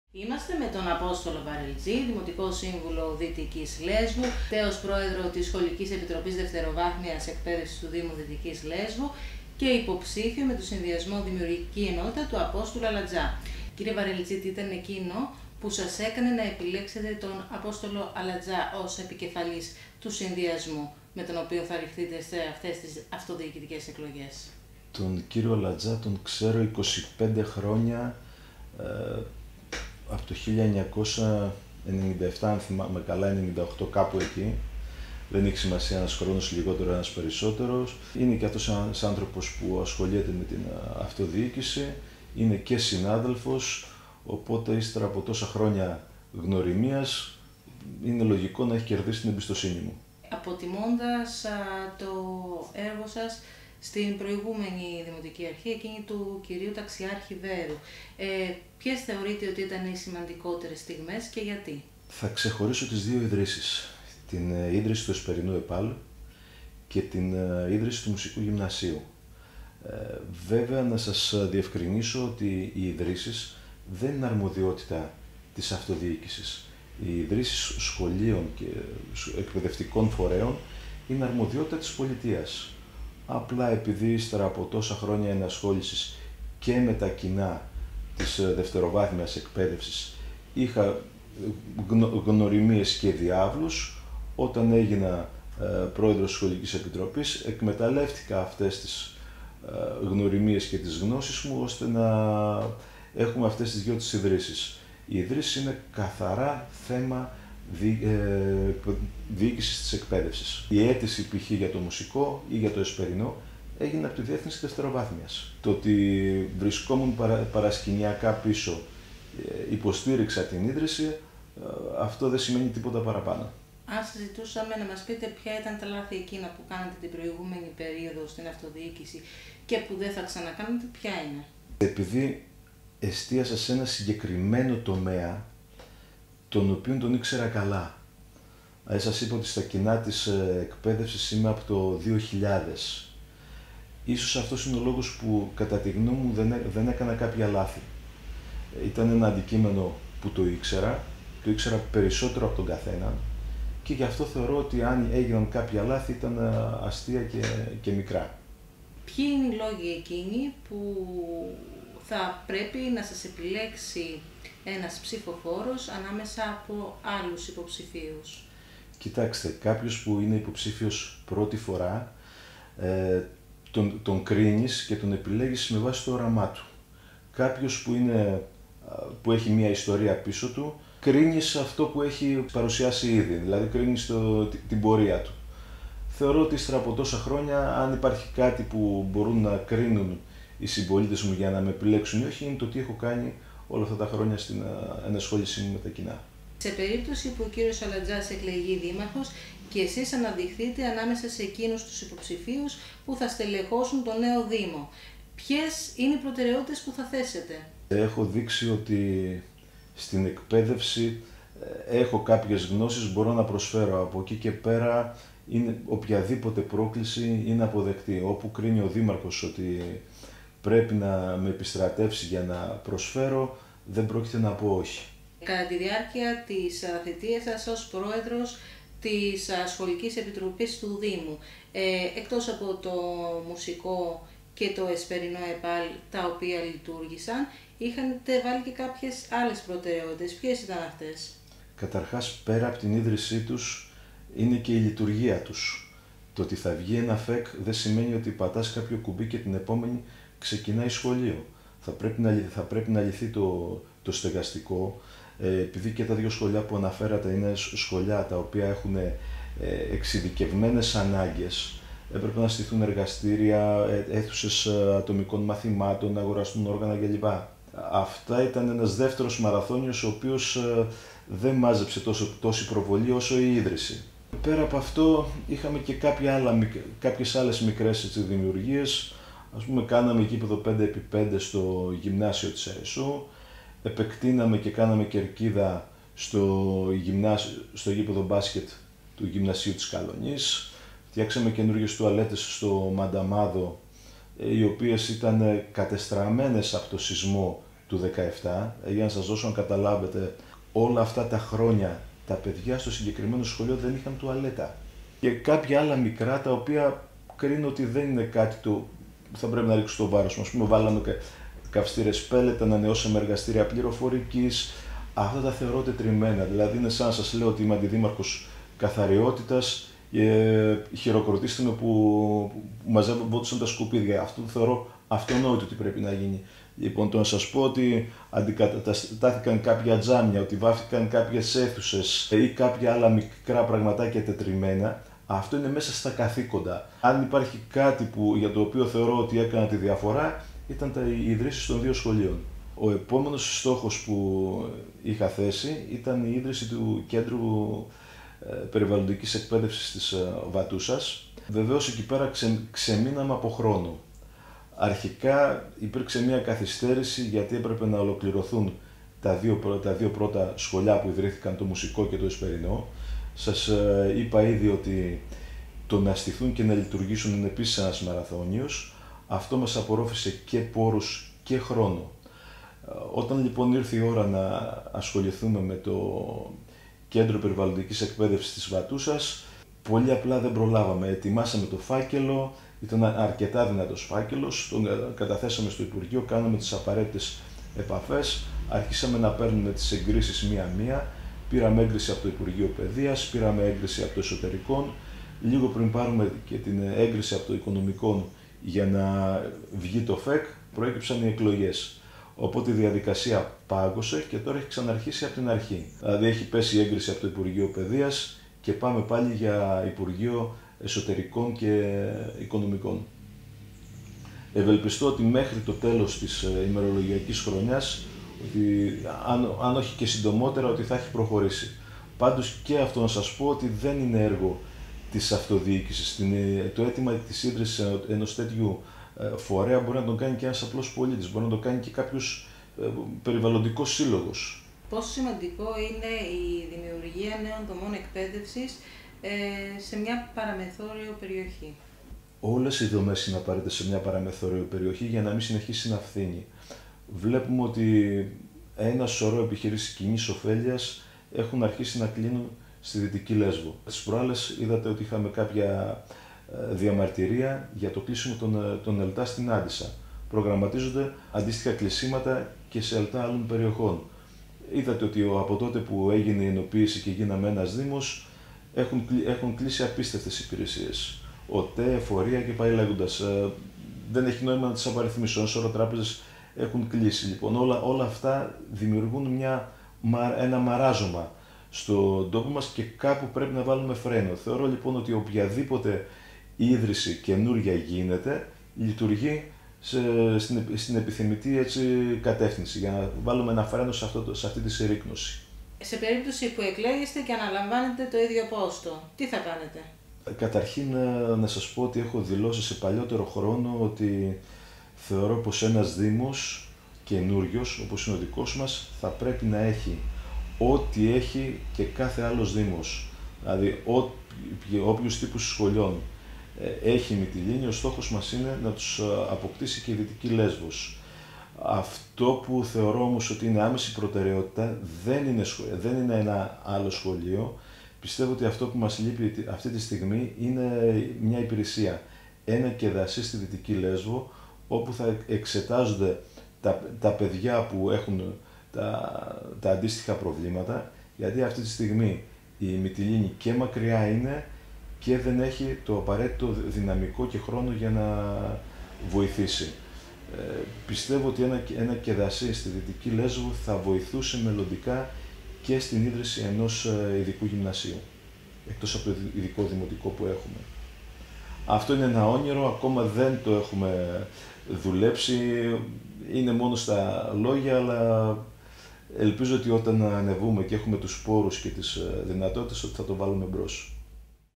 Βαρελτζής: «Επόμενος στόχος η δημιουργία ΚΕΔΑΣΥ στην Δυτική Λέσβο»
Υποψήφιος Δημοτικός Σύμβουλος με τον συνδυασμό «Δημιουργική Ενότητα» του Απόστολου Αλατζά, ο Απόστολος Βαρελτζής